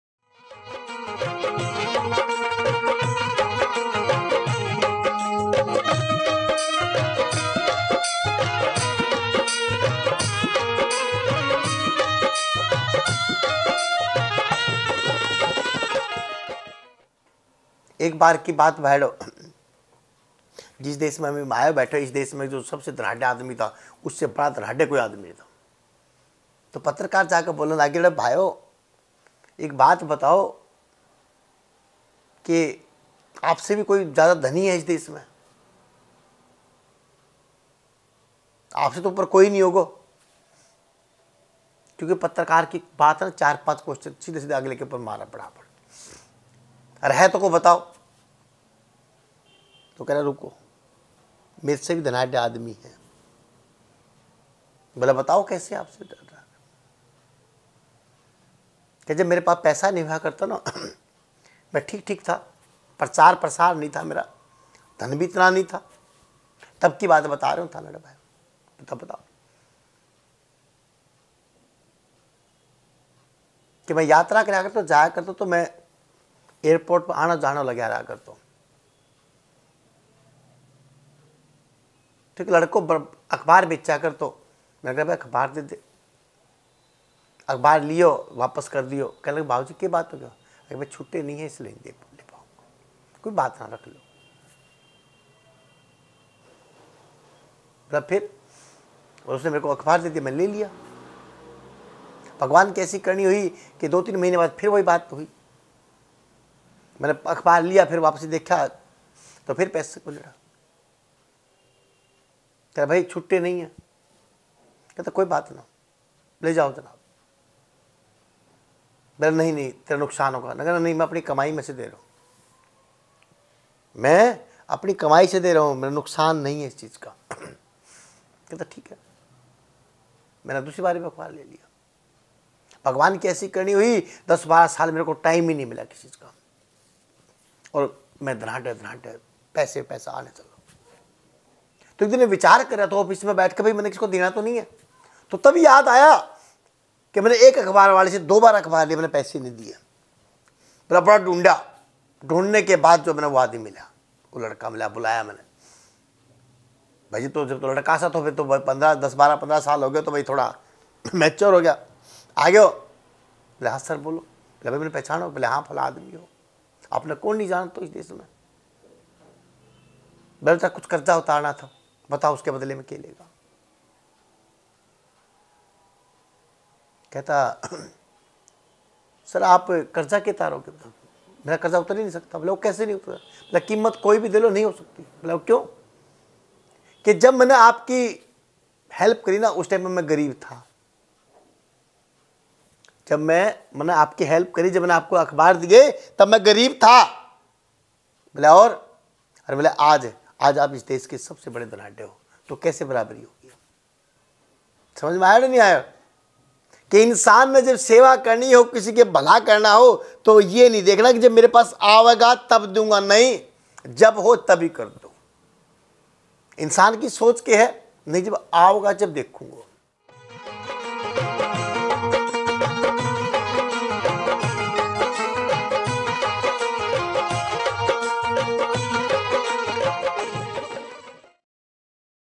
दान करने का कोई समय नहीं होता
"जब सेवा करने की इच्छा हो... जब किसी का भला करना हो... तो कर दो, सुकून मिलेगा।"

बाबाजी यहाँ पर समझा रहे हैं कि सेवा करने का, किसी का भला करने का, कोई समय नहीं होता है।  जब चाहो, कर दो।  अपने सीमित धन में से दान करो या कोई शुभ कार्य करो तो वह हमारे द्वारा किया पुण्य कर्म होता है।

जैसे, इस धनाढ्य सेठ के साथ हुआ।  यह अपने देश का सबसे धनी सेठ था।  एक पत्रकार ने इन सेठजी से पूछा, “क्या इस देश में आपसे भी कोई धनी व्यक्ति है?”  सेठजी ने कहा, “एक लड़का है,  मुझसे भी ज्यादा रईस।  जब मैं मध्यम वर्गीय परिवार का सदस्य था,  मेरा इतना नाम नहीं था और मेरे पास धन और दौलत नहीं थी।  तब मैं अक्सर काम के सिलसिले में हवाई यात्रा किया करता था।  एक दिन, मैंने हवाई अड्डे पर एक लड़के से अख़बार खरीदी। खुल्ले पैसे न होने के कारण, मैं उसे पैसे न दे सका।  दो-तीन महीने के बाद फिर से ऐसा ही हुआ।  आज मैं उस लड़के के दो अख़बार के पैसों का कर्ज़दार हूँ, इसलिए वह लड़का मुझ से ज़्यादा धनी है।

उस लड़के का पता लगाया गया।  अब वह नौजवान हो गया था।  जब सेठ जी ने उस नौजवान लड़के को उसके दो अख़बार की कीमत देनी चाही तो उस लड़के ने पैसे लेने से इन्कार कर दिया और उसने जो कारण बताया वह यह पूर्णतया साबित करता है कि दान करने के लिए हर समय सही है।

Being wealthy is not the measure of material worth; it is the good deeds you do for others.  Good deeds should be done with good intention, not for attention.  The smallest act of kindness is worth more than the grandest action that demands attention.  Helping others should come from your heart, not something you have to do to show off to others.  There is no correct or perfect time to do good for others.  Using the resources you have at hand, your pure and selfless deeds will leave an imprint in others’ hearts.

बालाजी दल🙏🏻
सेवा   धैर्य   विश्वास